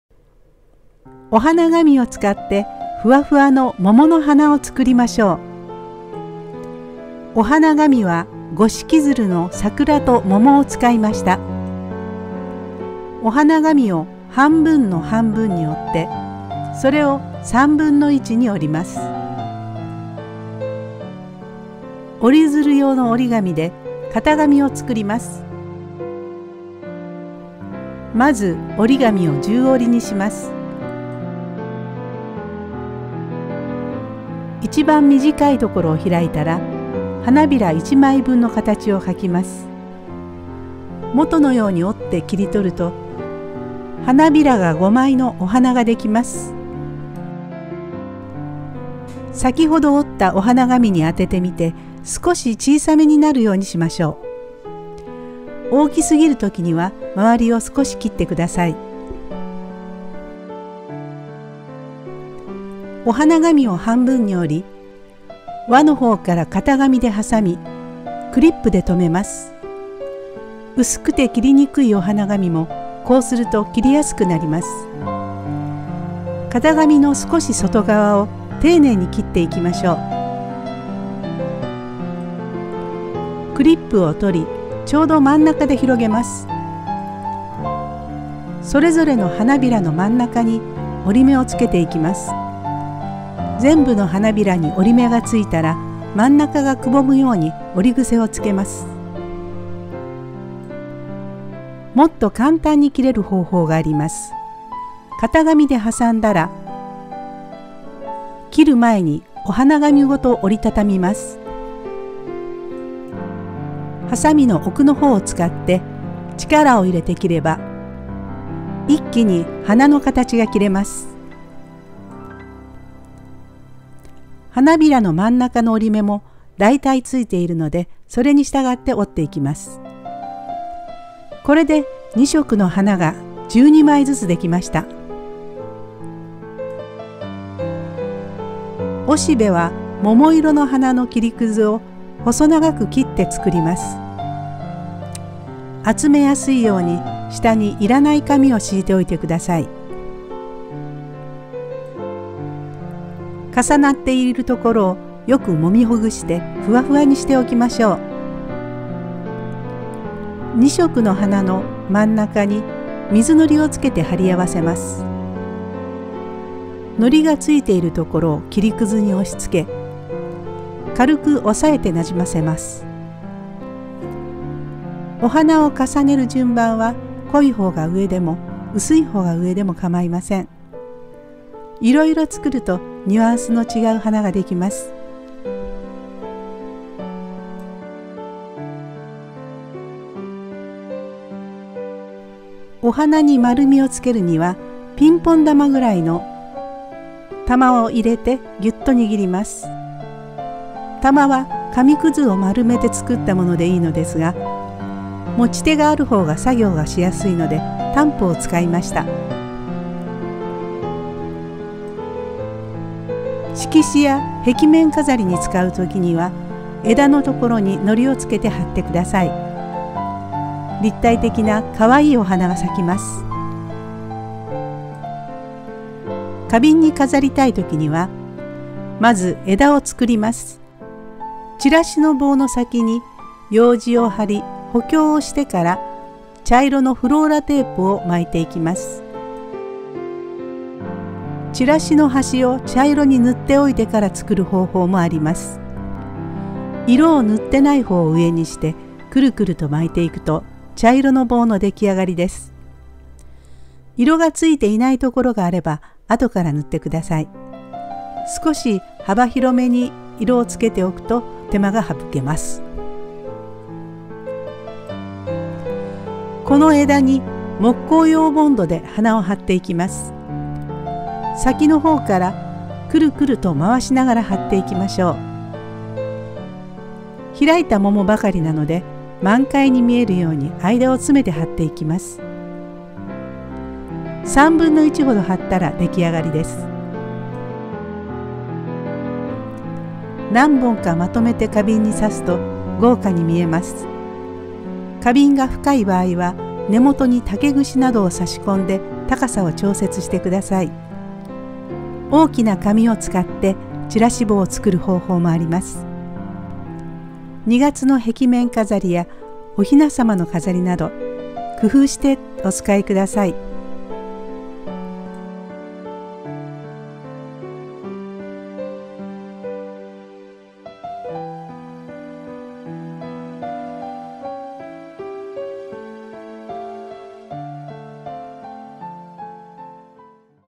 kimie gangi お花紙で作る「桃の花」
手分けしてたくさんの花を作っておけば、一気に壁面を桃源郷のように飾ることができます。
壁に直接貼ることも、枝に貼って花瓶に飾ることもできます。

材料
　桃の花‥お花紙（五色鶴）さくら・もも
　　　　　折り鶴用折り紙１枚（型紙用）
　枝‥チラシ棒、フローラテープ（茶色）
用具
　筆記具、はさみ、クリップ、液体のり、木工用ボンド

蕾と若葉の作り方は、こちらをご覧ください。

0:00　オープニング
0:06　お花紙準備
0:21　型紙作り
1:00　花型に切る
2:12　おしべ準備
2:39　花パーツ組合
3:12　丸み付け
3:32　飾り方(平面)
3:45　枝作り
4:26　花を付ける
4:50　生ける